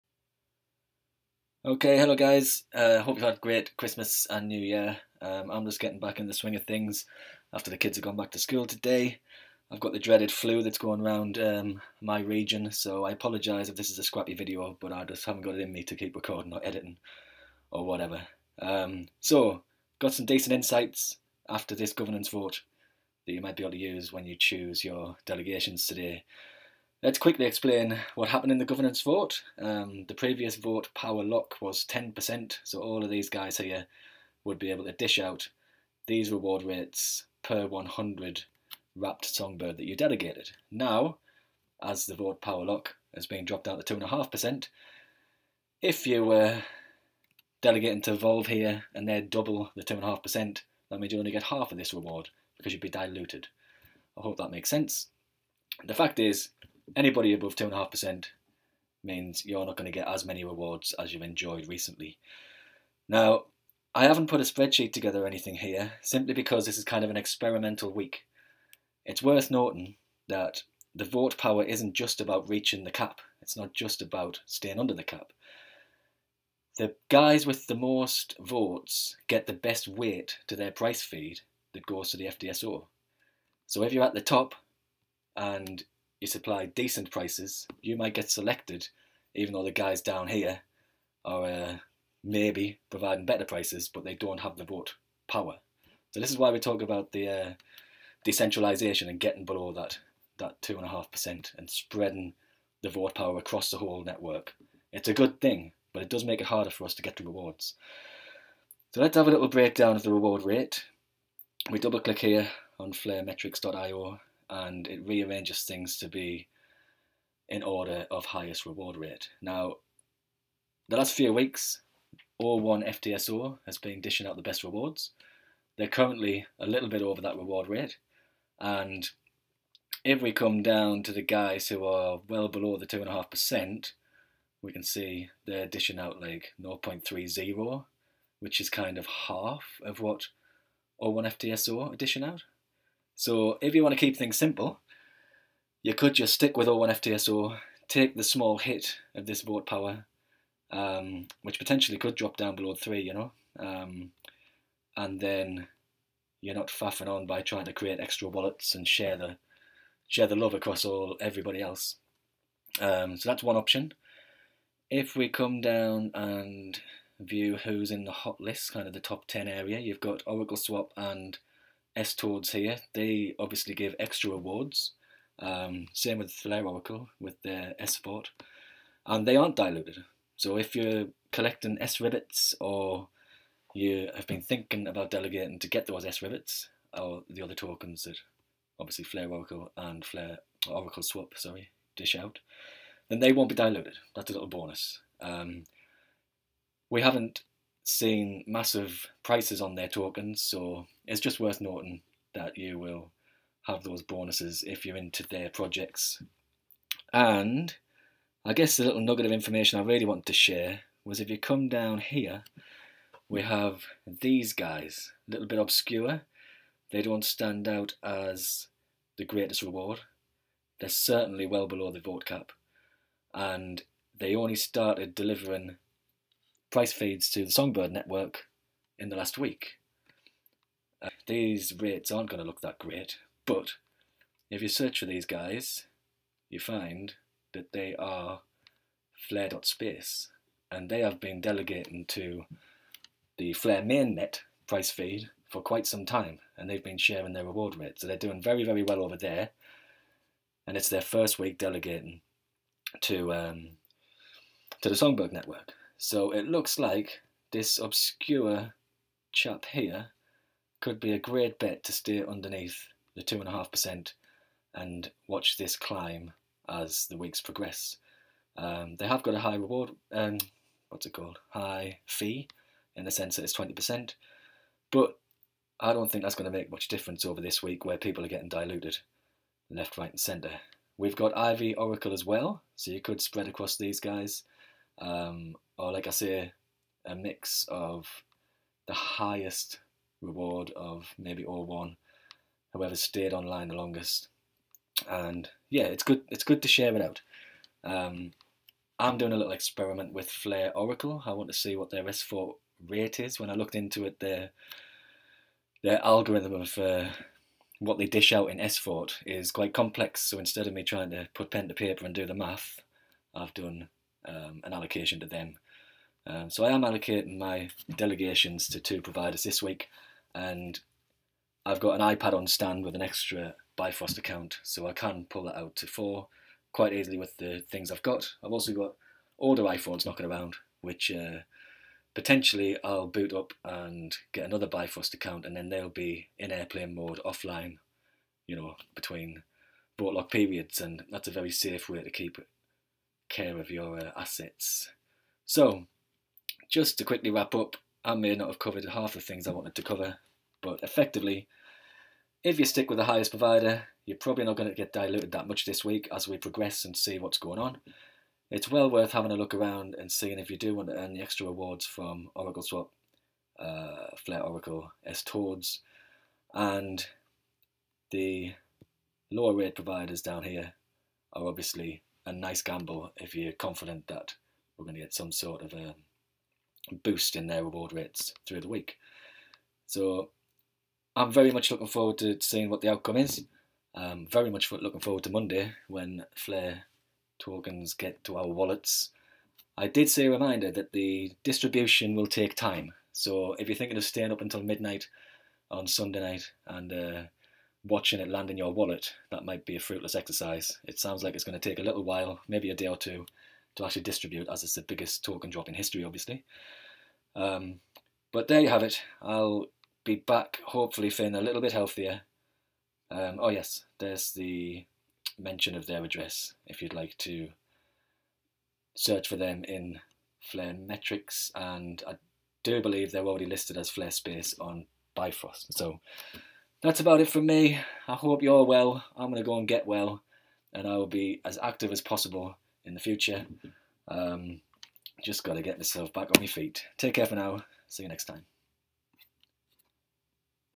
WSGB Delegation Day! Best options with a 2.5% vote cap on Songbird Network?!?!
The governance proposal passed, which is great for the network, but we now have the tricky task of seeking rewards without being diluted... Here's what's on the radar this week...